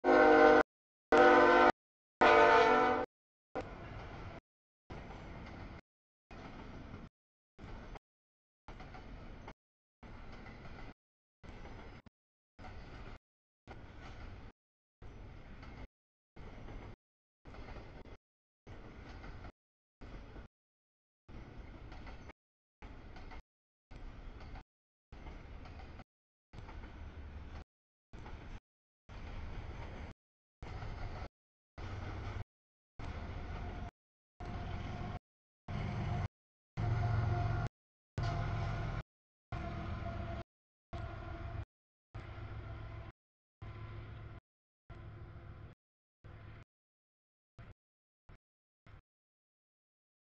3- 25-2019 07:17 sb stack CSX 3417 DPU 984 Q158
00:27 sb stack CSX Q162
00:34 nb stack trailers CSX Q009
00:46 nb stack CSX Q159
01:09 nb stack CSX Q157
03:08 sb mix CSX 732 Q409
03:48 sb tanks triple BNSF K160
03:54 nb auto rack KCS BCOL Q261
04:12 nb mix, green trash CSX Q438
06:14 sb stack trailers CSX Q010
06:39 nb stack trailers CSX DPU Q003
07:17 sb stack CSX 3417 DPU 984 Q158
07:52 sb stack trailers CSX 5483, 5461 Q008
07:58 nb mix CSX 890, 26, 8823, 3313 Q190
08:49 nb auto rack CSX 3067, 3184 Q253
09:54 nb nysw cut, CSX 754, 5377 GECX Q410
10:40 sb tanks CSX 290, 84 K682
11:01 sb auto rack CSX 575, 208 Q234
11:38 nb mix, epic  trash CSX 316, 126 Q434
11:52 nb empty's BNSF 8217, 6580, 4628 K141
12:46 sb green trash, mix CSX 5440, 203 Q439
14:26 sb auto rack CSX 3129 DPU 3364 Q262